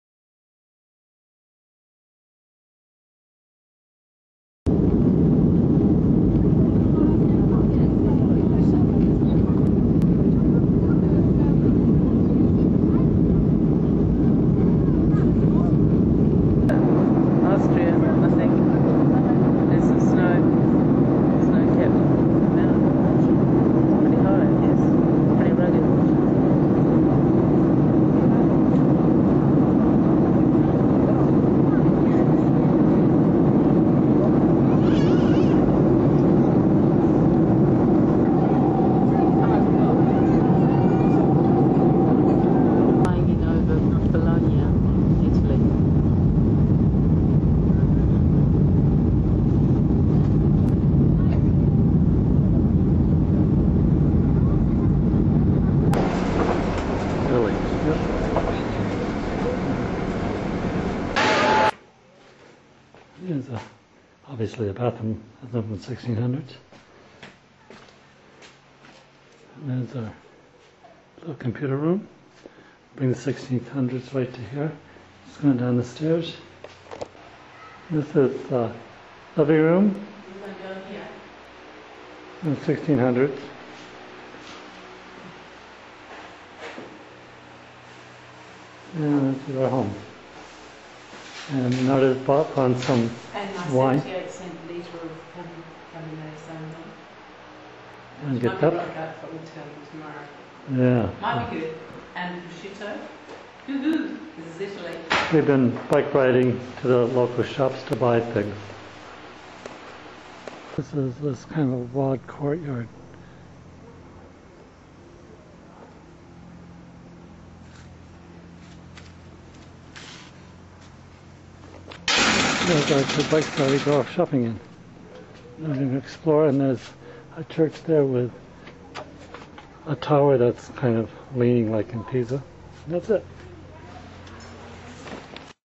Ferrara Italy
Ferrara is a city in Emilia-Romagna, northern Italy, capital city of the province of Ferrara. on the Po di Volano, a branch channel of the main stream of the Po River, Castello Estense, Piazza Trento e Trieste Corso Ercole I d'Este The church of San Giorgio Teatro Comunale (Community Theatre). more at ournews.mobi/album.htm

Terrell Neuage
writer: Terrell Neuage